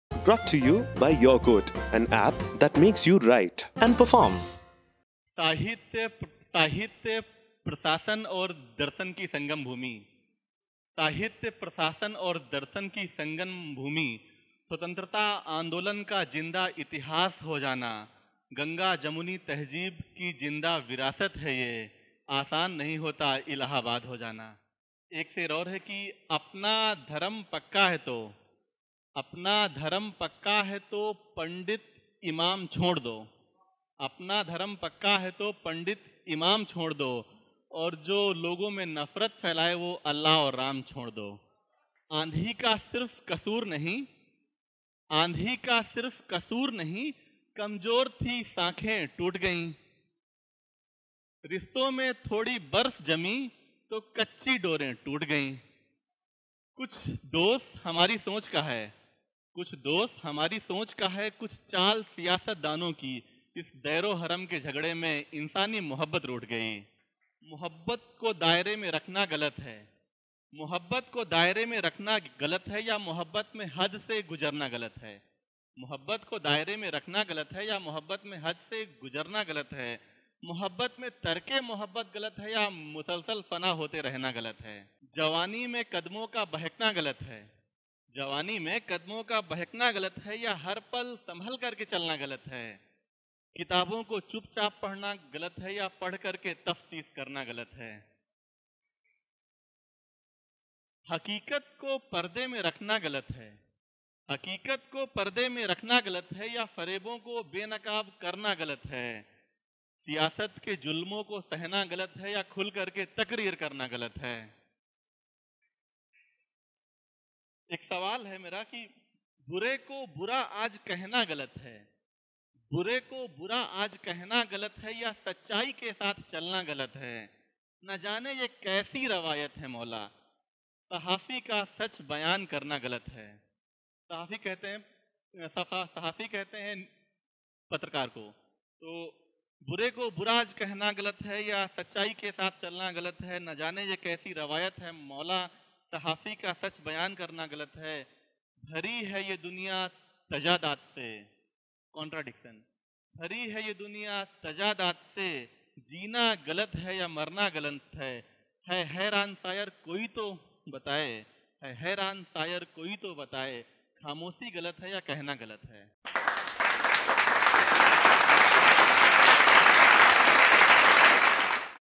'Kya Galat Hai' by Himanshu | Hindi Poetry | YQ - Noida (Open Mic 13)
Date: 23rd December 2018
Venue: 91springboard, C-2, Sector 1, Noida